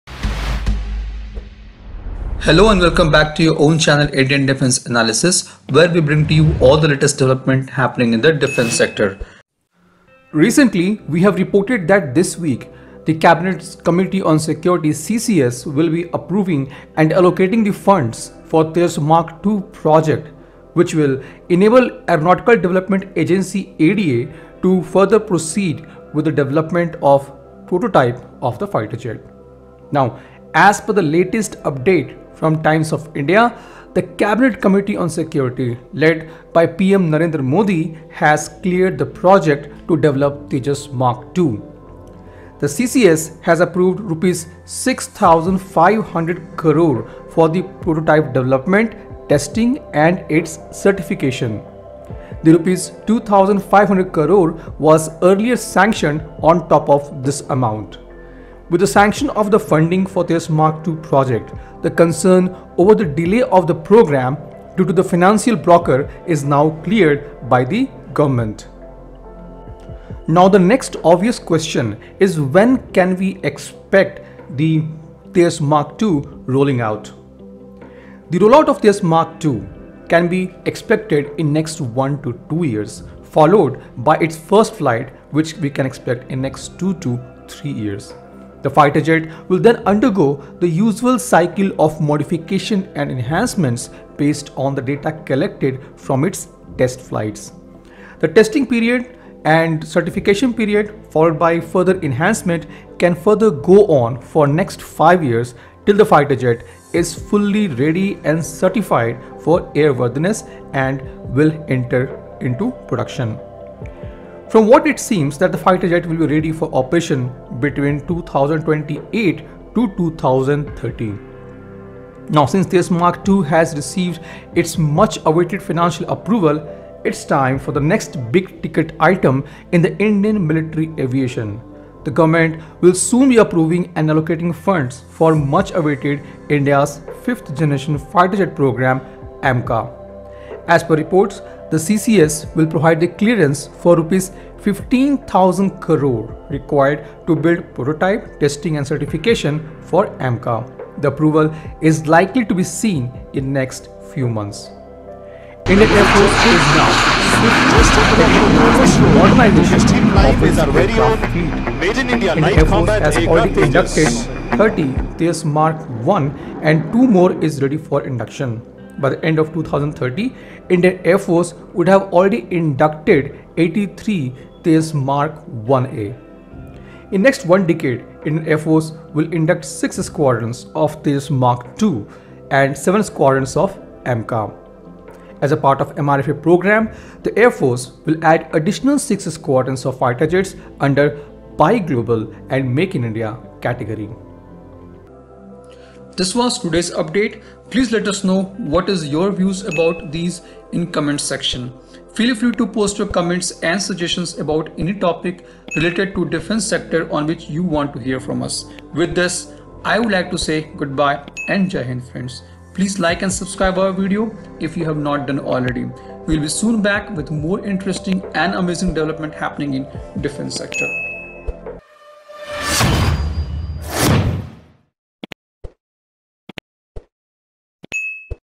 Government approves Rs6,500 crore for Tejas Mark2 | 23 Squadron of new fighter jets
Rs 6,500 crore approved for Tejas Mark2

Recently we have reported that this week CCS will be approving and allocating the fund for the Tejas Mark2 project which will enable ADA to proceed with the development of Prototype of the fighter jet. As per the latest update from the TOI, the CCS led by PM Narendra Modi has cleared the project to develop Tejas Mark-2. CCS has approved Rs 6,500 crore for the prototype development testing and its certification. Rs 2,500c crore was sanctioned earlier on top of this amount. With the sanction of the funding for the Tejas Mark2 project, the concern over the delay of the program due to financial blocker is cleared now by the government. We can expect the rollout of Tejas Mark-2 in 1-2 years and its first flight in 2-3 years. The fighter jet will then undergo the usual cycle of modifications & enhancements based on the data collected from its test flights. The testing period can go on for 5 years till the fighter jet will be fully certified for airworthiness and ready for operation. From what it seems that the fighter jet will be ready for operation between 2028 to 2030.

Rs 15,000 crore for AMCA in few months

Now, since the Tejas Mark2 has received its much-awaited financial approval, it’s time for the next big-ticket item in the Indian military aviation. The government will soon be approving & allocating the funds for the much-awaited India’s 5th generation fighter jet program AMCA. As per reports the CCS will provide the clearance for Rs 15,000 crore required to build the prototype, testing & certification of AMCA. The approval is likely to be seen in next few months.

23 Squadron of new fighter jet

Indian air force is now swiftly heading towards the modernization of its aircraft fleet. IAF has already inducted 30 Tejas Mark1 and 2 more is ready for induction. By the end of 2030, IAF would have already inducted 83 Tejas Mark1A. In next one-decade IAF will induct Six squadrons of Tejas Mark2 & 7 squadrons of AMCA. As a part of MRFA program, the air force will add additional 6 squadrons of fighter jet under buy global and make in India category.